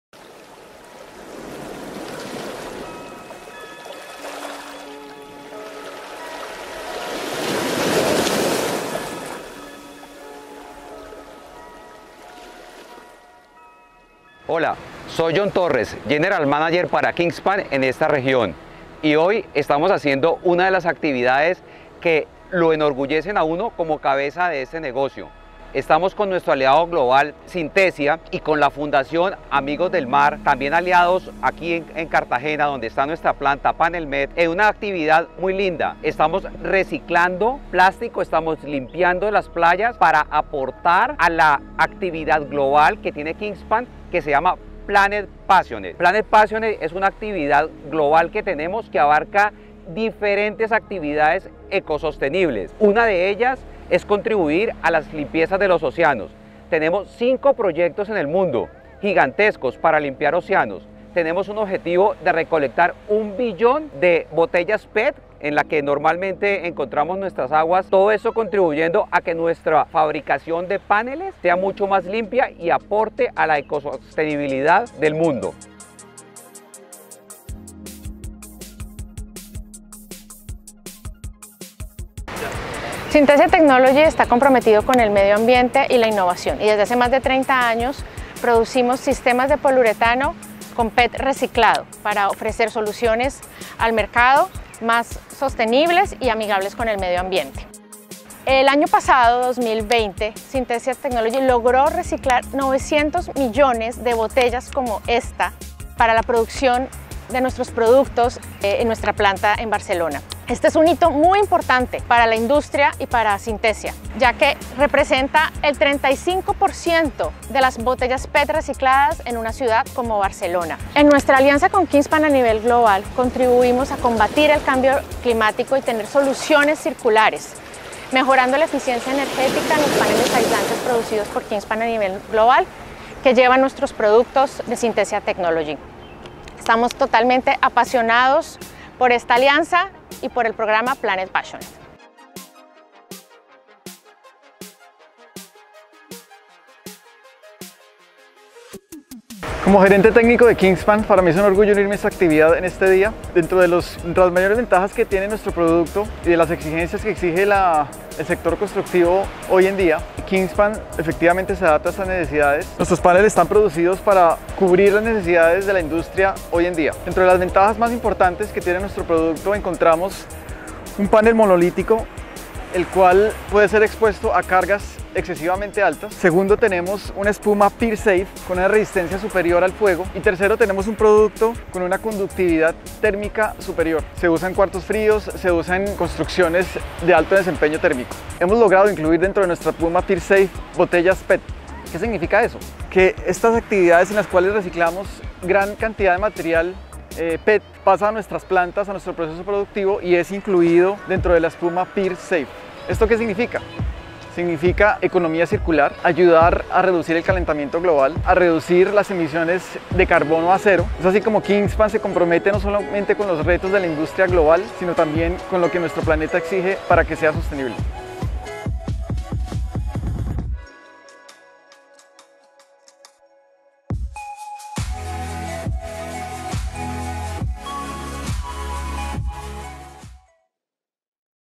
Recogida de plásticos de Synthesia Colombia en Cartagena de Indias
En la acción de voluntariado ambiental de recogida de plásticos de Synthesia Colombia en Cartagena de Indias (Colombia) han participado 40 personas, esto ha supuesto la recogida de 56 kilos de residuos plásticos para contribuir con la sostenibilidad y la conciencia ambiental.